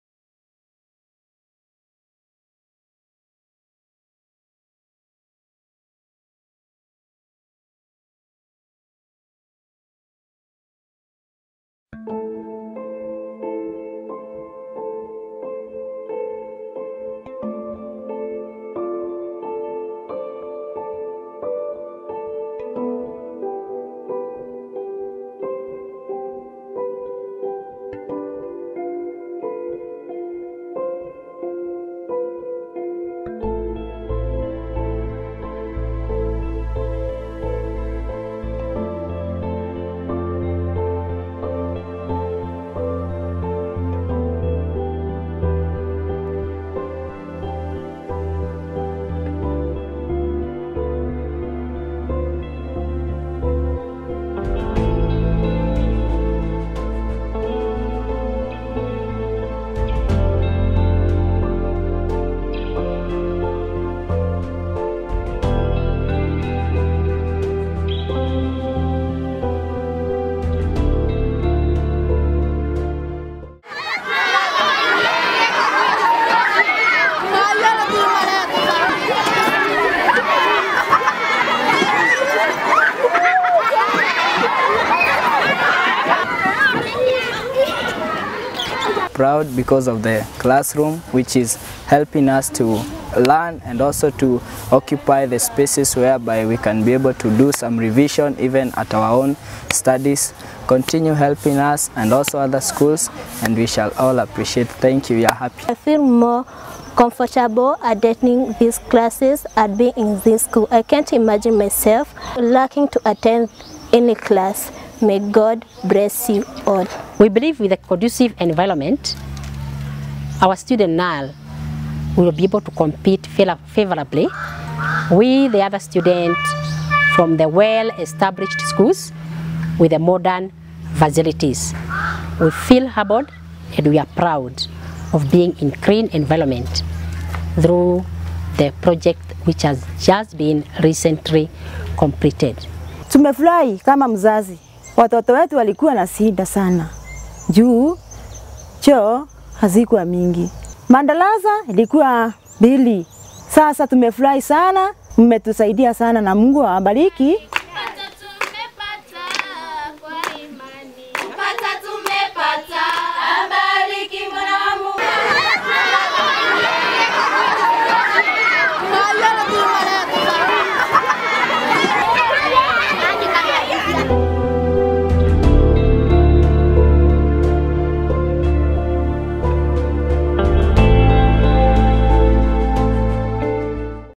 Githage School Impact Video
TGUP, with the help of two generous donors in California, built a new classroom, playground, latrines, water catchment system and installed electricity for the Githage School in Kenya.